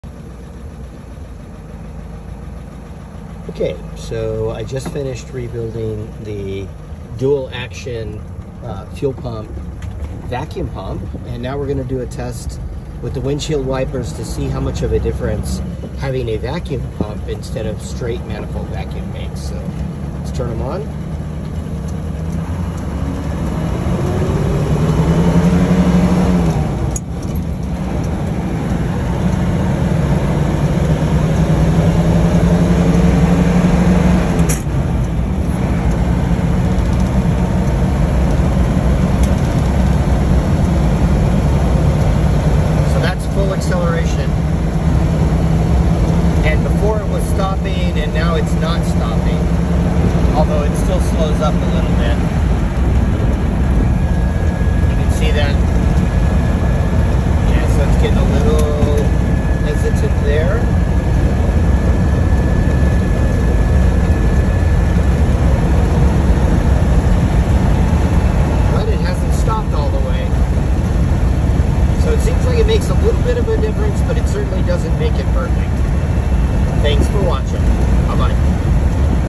1936 Chrysler C8 Convertible Vacuum Wiper Performance with Factory Fuel/Vacuum Pump
Compare this video to the previous windshield wiper performance with straight intake manifold vacuum.